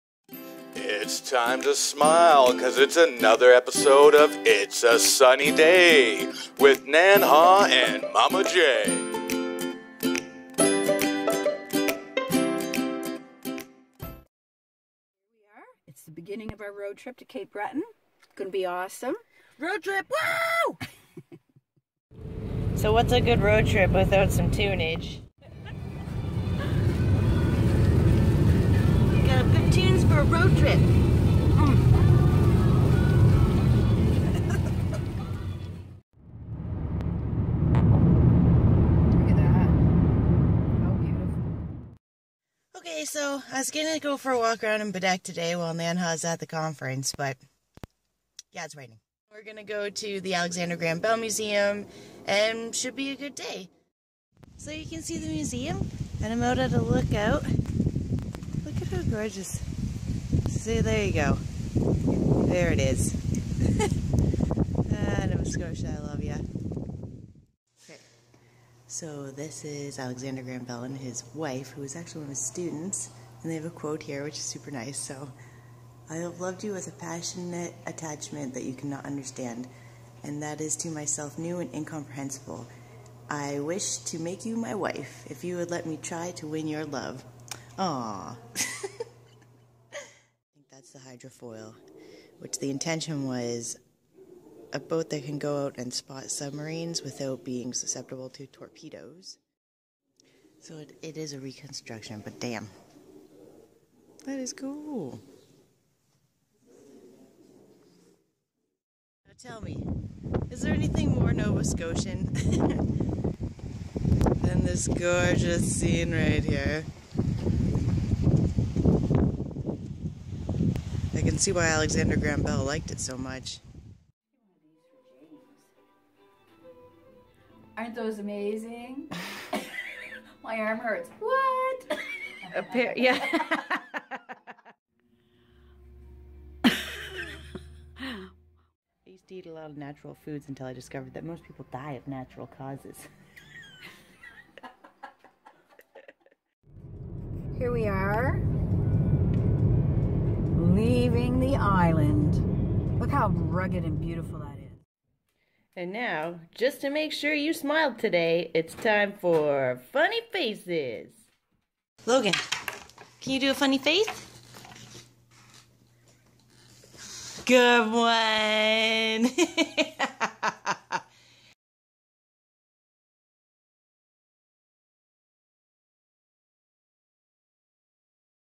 Episode 8 - Cape Breton Road Trip
Come with us as we road trip to the beautiful Baddeck. While Nanha is at a conference Momma J checks out the Alexander Graham Bell Museum then we go shopping at the Red Barn gift shop. It was a great road trip and we are excited for you to see our video.